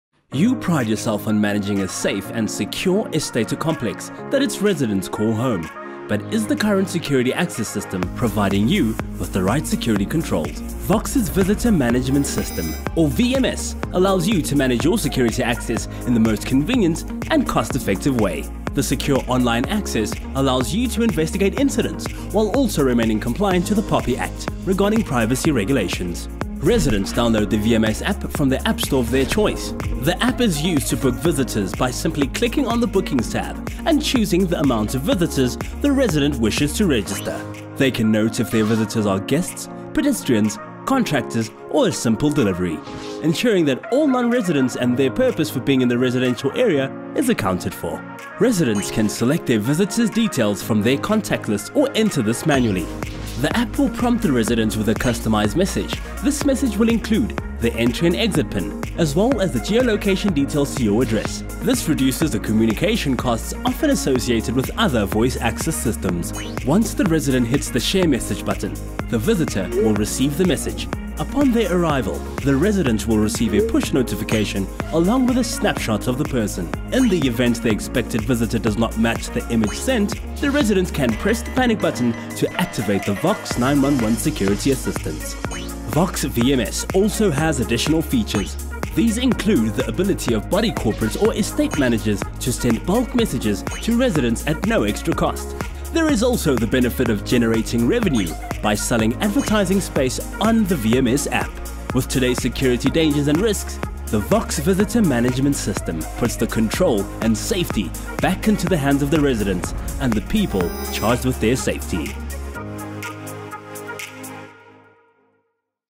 Vox Visitor Management System
Vox’s Visitor Management System, or VMS, allows you to manage your security access in the most convenient and cost-effective way.